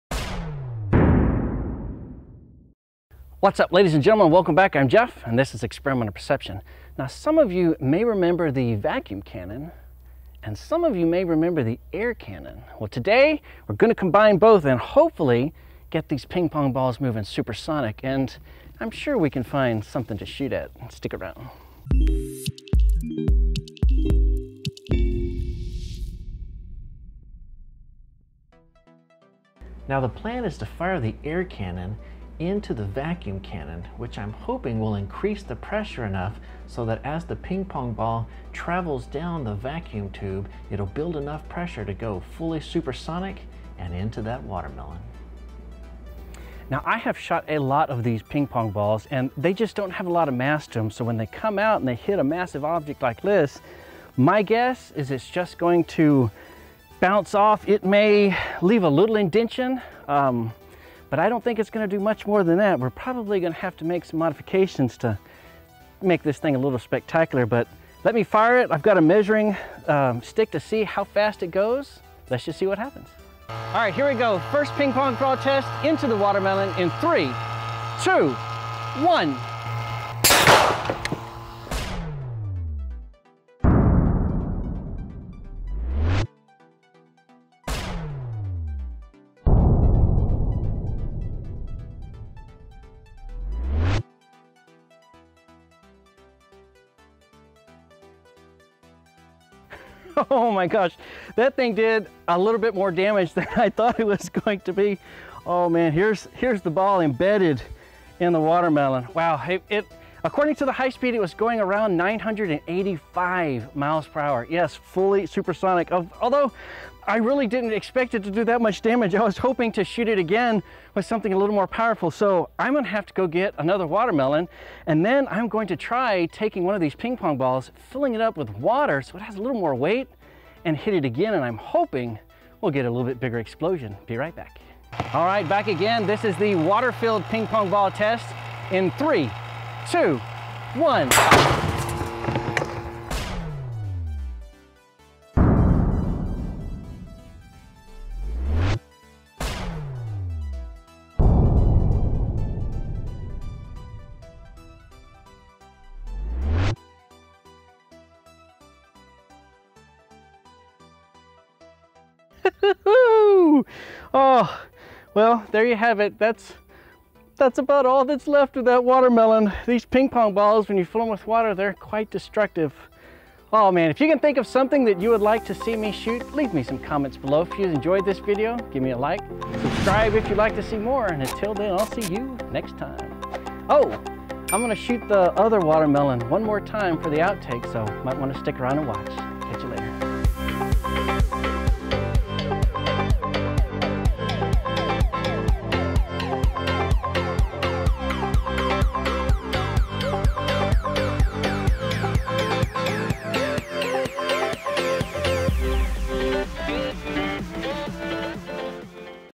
Super Sonic Ping Pong Balls vs Watermelons in super slow motion.
Today these ping pong balls are going super sonic! Yep! In this experiment I combine the vacuum cannon with the air cannon to greatly increase the speed of those little orange balls. To make it even more fun, I shoot them at watermelons and capture it all in super slow motion.

Main Camera
Slow Motion Camera